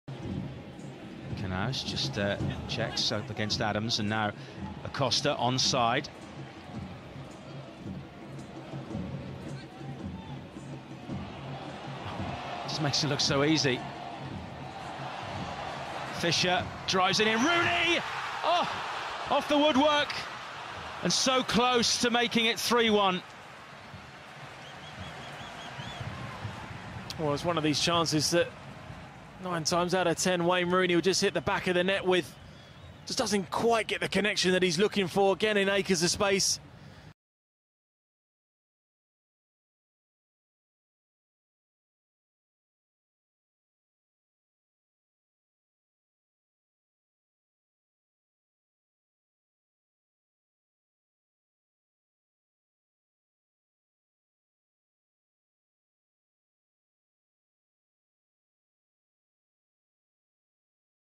Wayne Rooney Hits The Crossbar 16/09/2018
Wayne Rooney kick the ball in the post during the draw game 3-3 between D.C. United and New York Red Bulls.

D.C. United vs New York Red Bulls, MLS, Wayne Rooney post hits vs New York Red Bulls.

16/09/2018